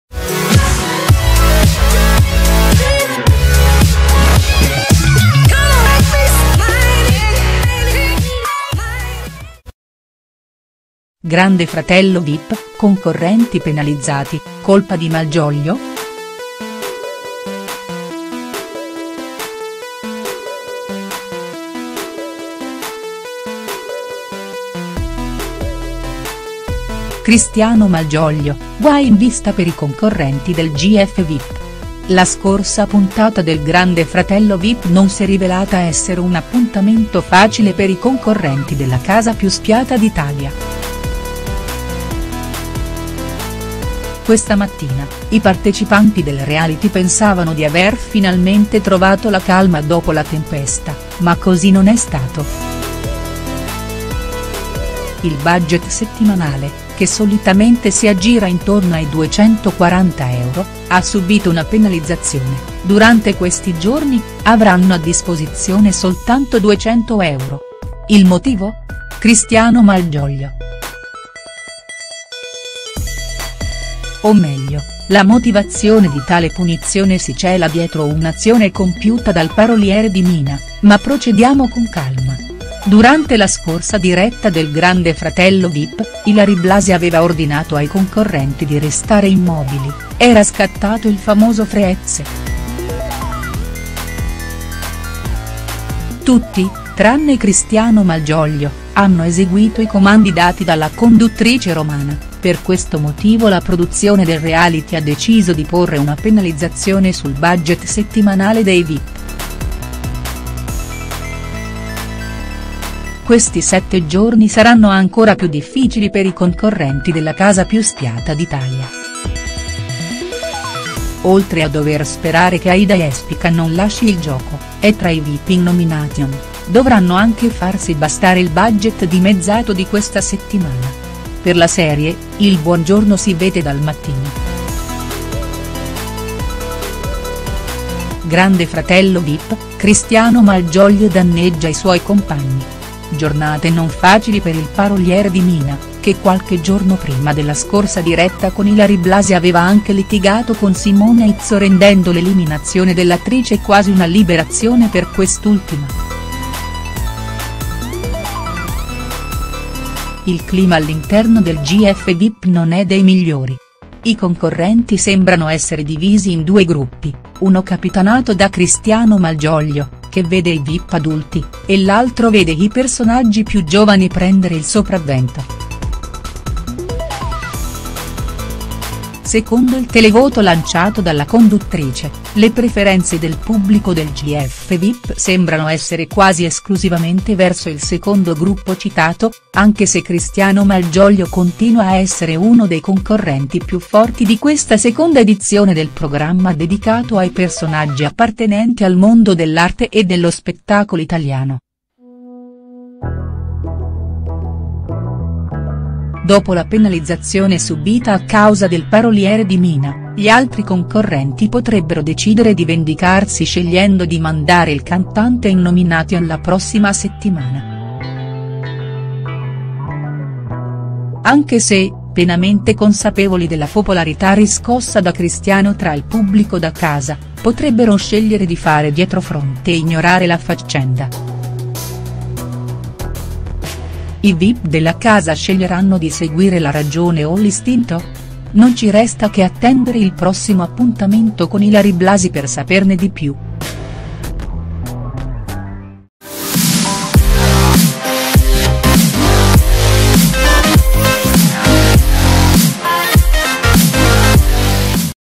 Grande Fratello Vip, concorrenti penalizzati: colpa di Malgioglio?.HD
Cristiano Malgioglio: guai in vista per i concorrenti del GF Vip La scorsa puntata del Grande Fratello Vip non si è rivelata essere un appuntamento facile